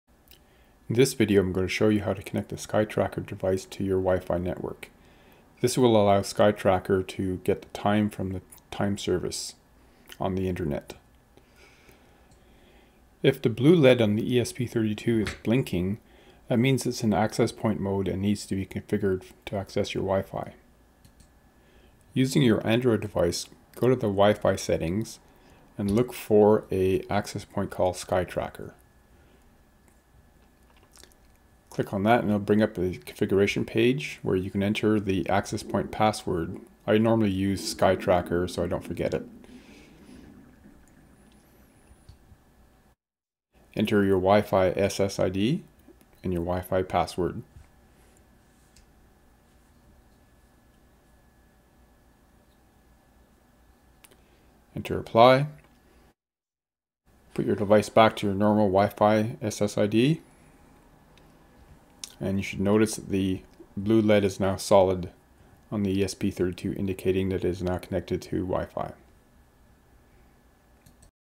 SkyeTracker Wifi setup using Access Point
This video shows you how to configure Wifi for the SkyeTracker Solar tacker.